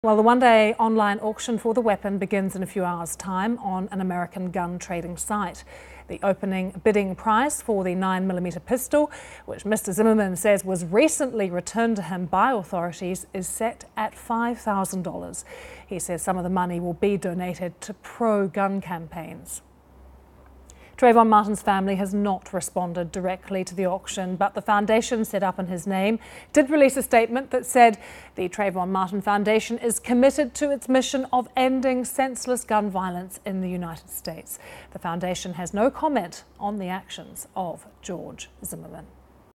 George Zimmerman to auction Trayvon Martin death gun - BBC News
A gun used to shoot dead unarmed Florida teenager Trayvon Martin is to be auctioned by the man who shot him, George Zimmerman. Mr Zimmerman, a neighbourhood watchman, was cleared over the death of the 17-year-old in February 2012 after saying he acted in self-defence.
He said he was selling the gun partly to raise funds to fight "Hillary Clinton's anti-firearm rhetoric".
The Martin family would not "comment on the actions of that person", they said.